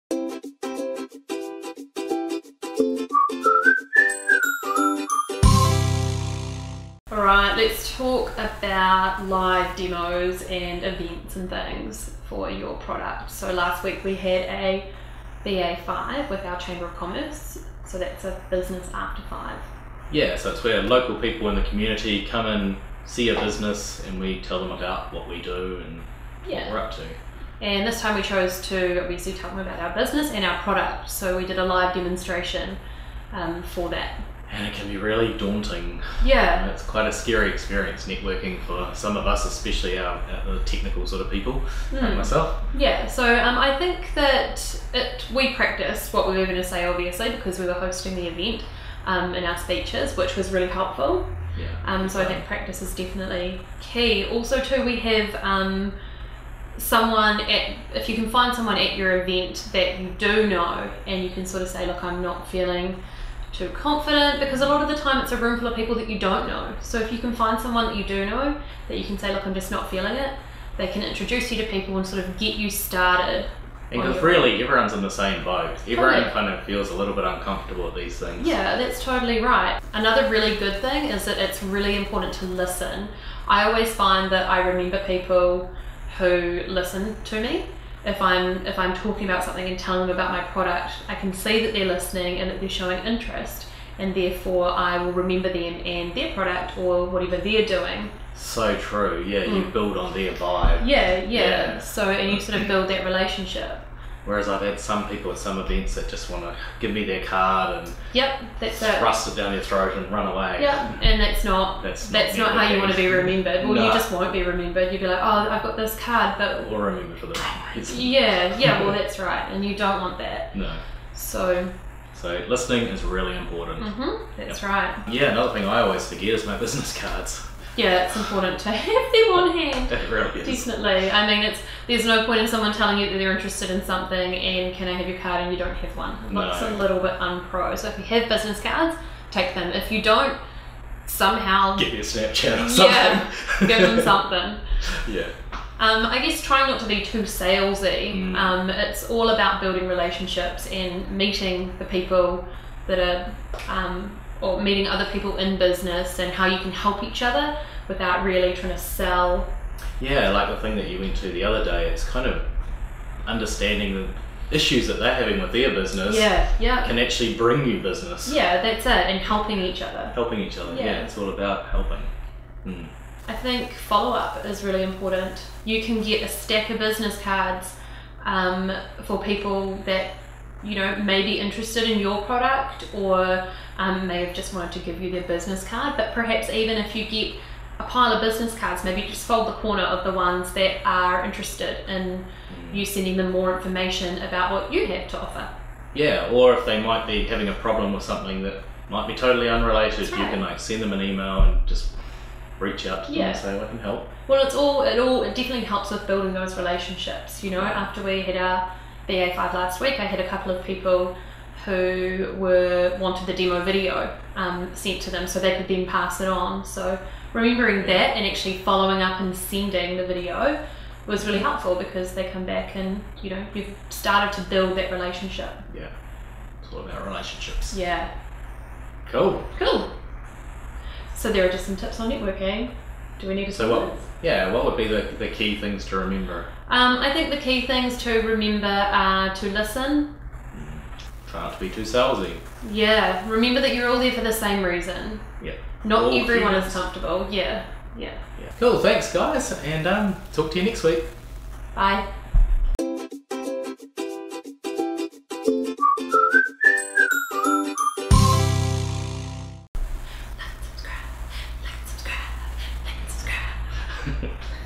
Giving A Live Demo Of Your Product | Professional Networking Tips
You've put your heart and soul into developing a product, and you're confident that it rocks! Now, the best way to get prospective buyers of that product to agree with you is through a Live Demo.

But to get there, you must first be 'found'. You'll be up against a multitude who'll be shouldering you aside, to SELL their product. Clearly, there's little point in trying to be another 'salesman'.

We have instead, forever relied on building relationships – how we can mutually benefit each other through collaboration. That's our Mantra, and that's what's got us this far. Here we share, from our own experience, a few professional networking tips you can use to market your product.

Watch my video "Looking To Build Website From Scratch? Here's How We Did It| Starting a SaaS App Grammin'"

Hit the 'Like' button if you found us helpful!!